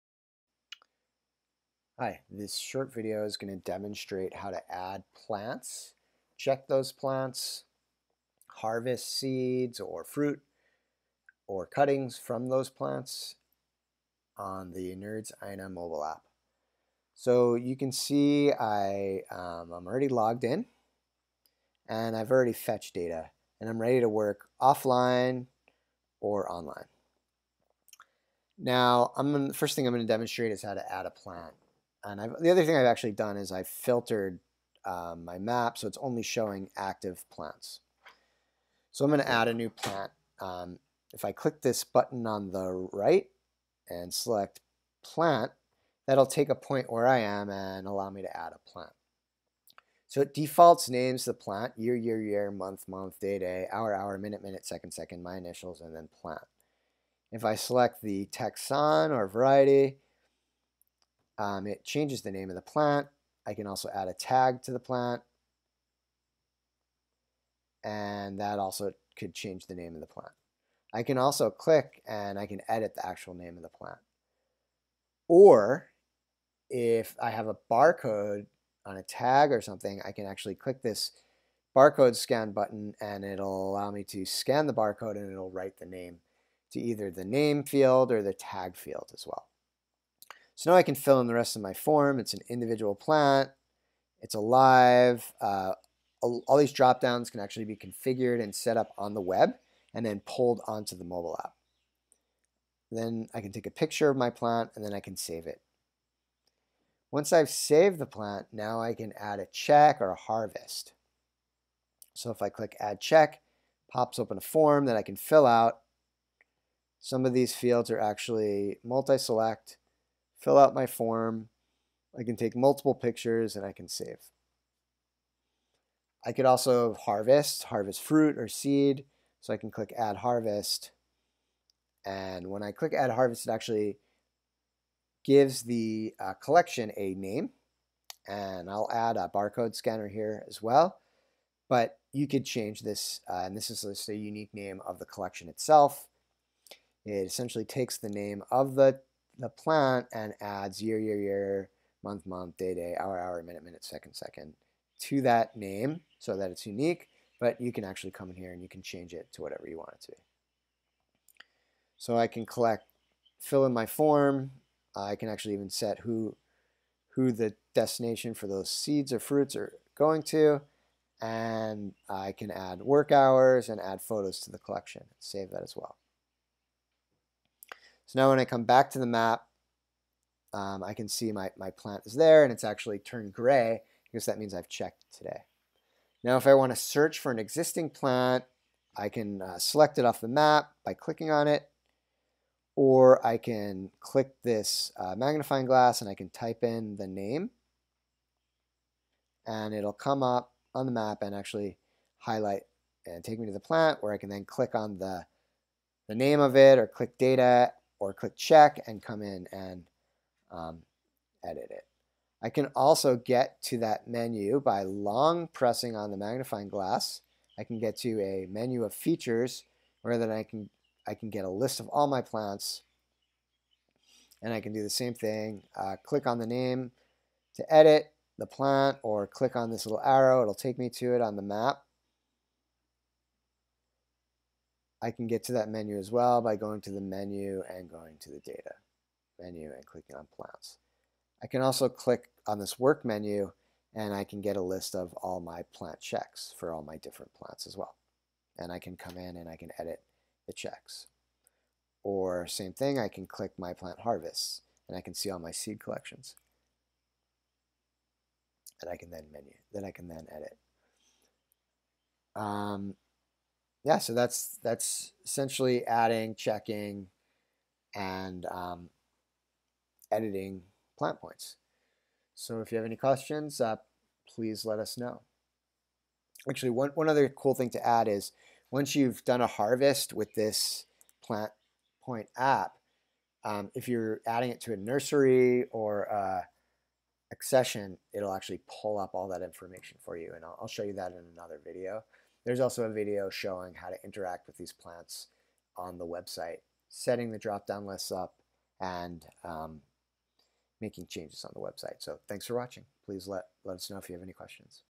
Plants on the NRDS Aina Mobile app - Checks, Planting, Harvest
A video that describes how to add plants, check them and harvest from them on the NRDS aina mobile app.